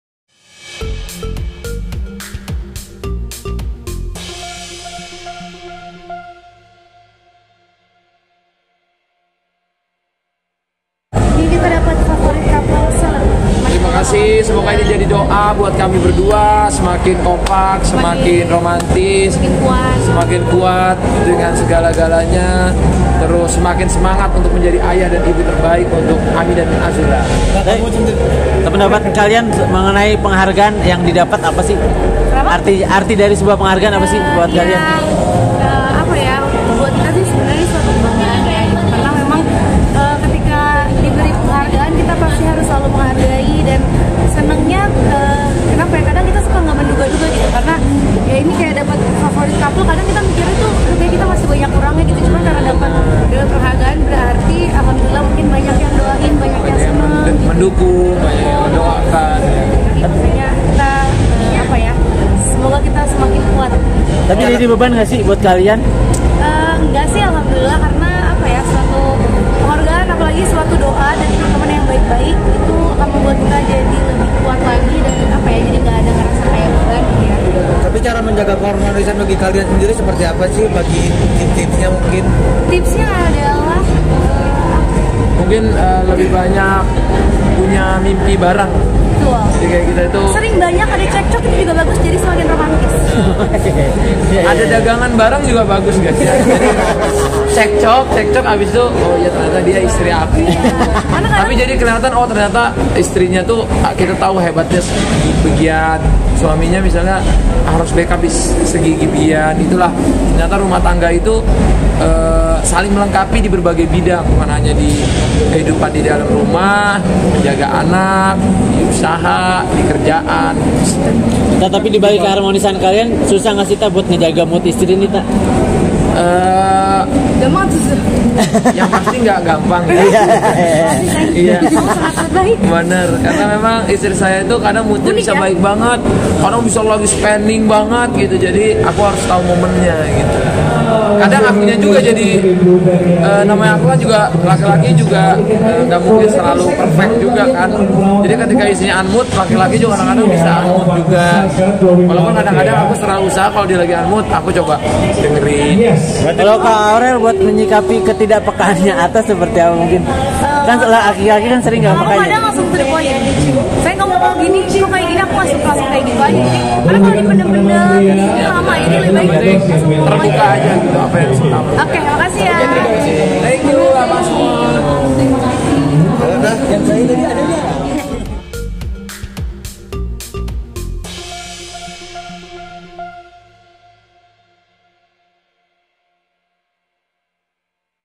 INI CARA ATTA HALILINTAR DAN AUREL MENGHADAPI CEK COK DALAM RUMAH TANGGANYA
Atta Halilintar dan Aurel Hermansyah kembali mendapatkan penghargaan pasangan selebriti terfavorit. Atta dan Aurel juga ceritakan cara mereka menjaga keharmonisan keluarga terlebih ketika sedanf terjadi cek cok antar keduanya.